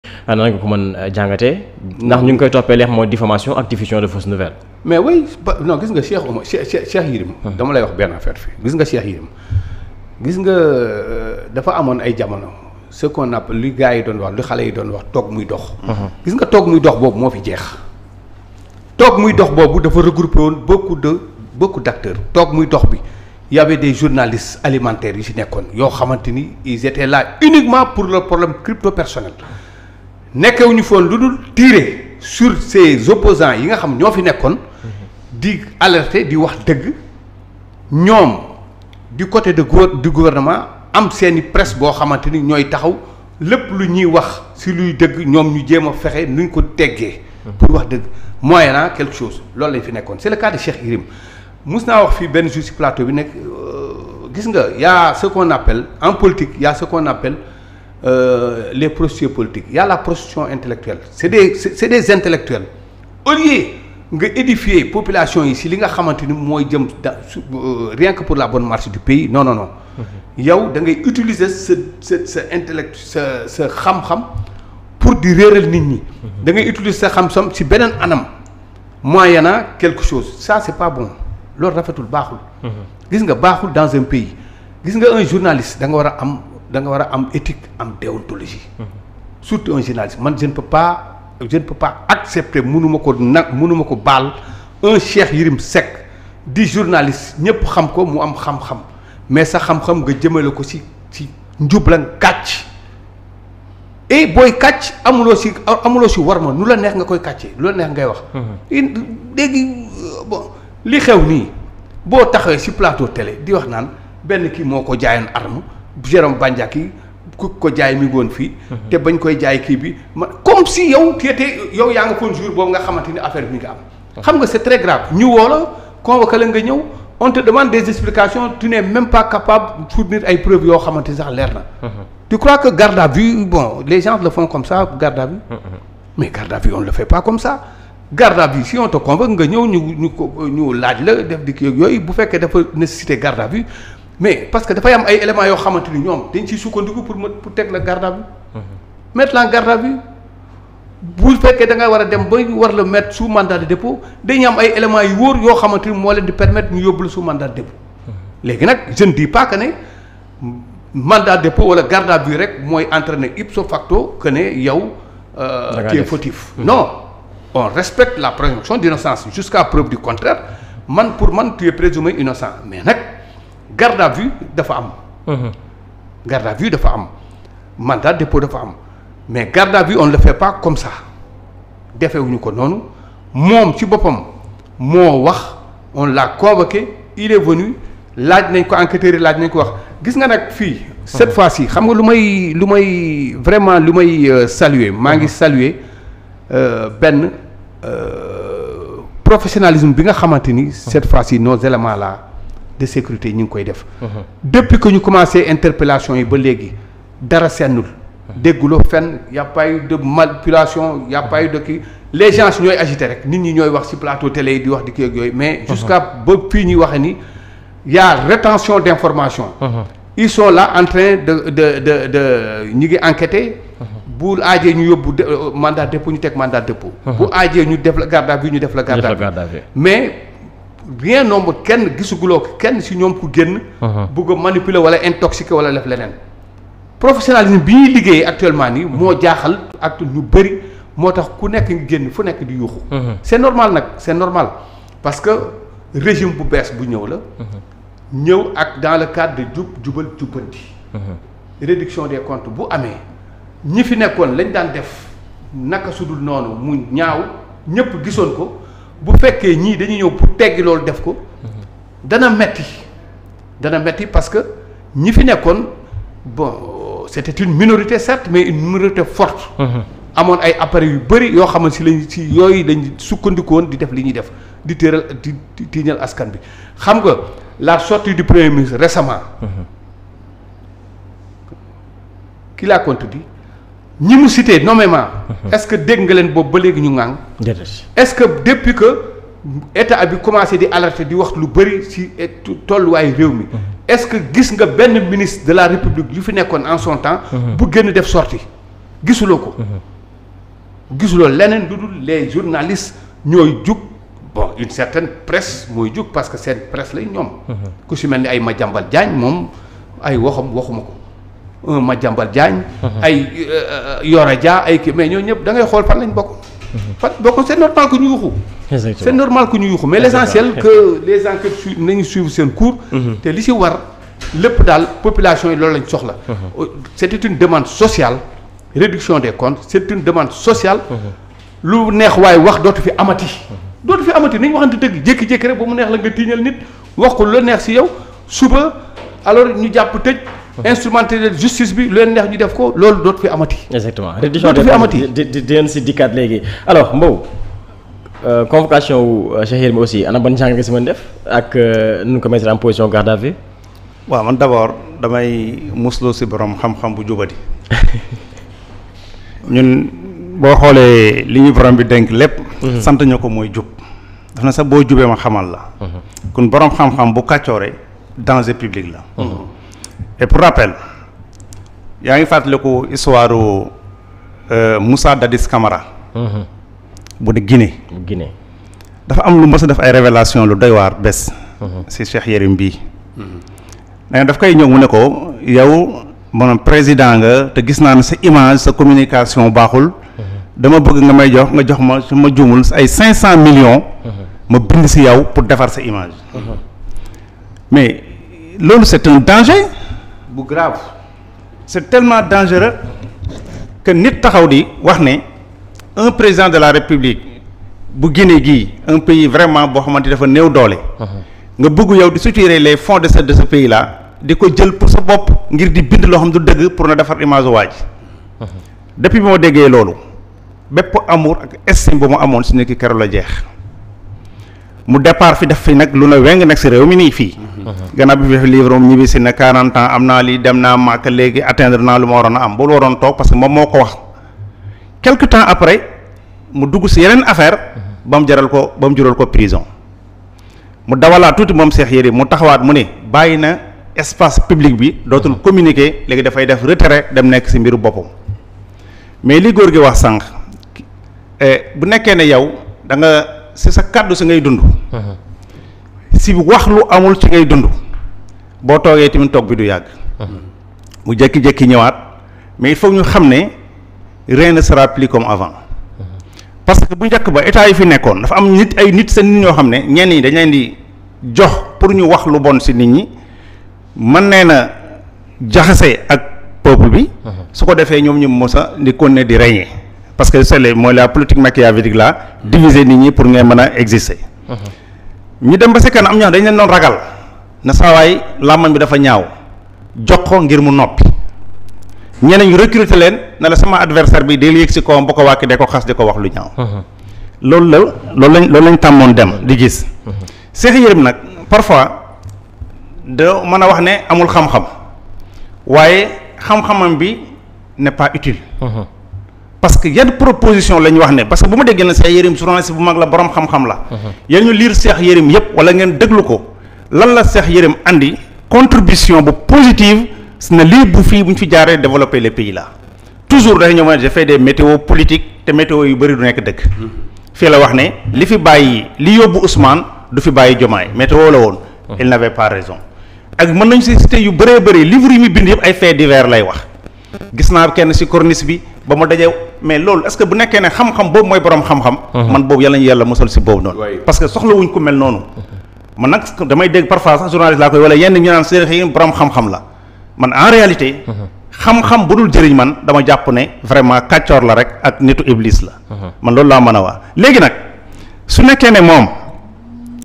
Première nuit de Cheikh Y Seck en prison: Alpha Diakhaté réagit après son arrestation «wakh mouy dok
Première nuit de Cheikh Y Seck en prison: Alpha Diakhaté réagit après son arrestation «wakh mouy dokh… »